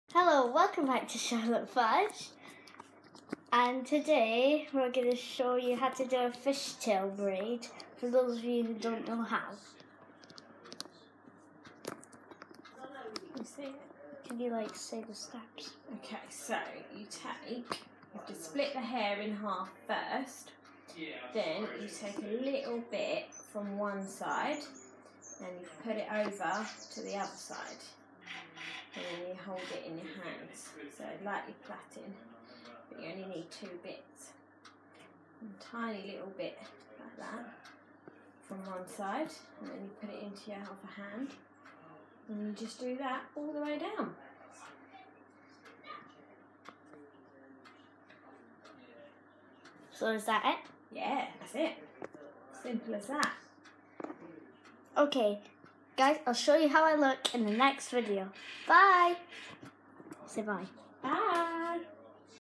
Fishtastic!
Holly is doing a fishtail braid on me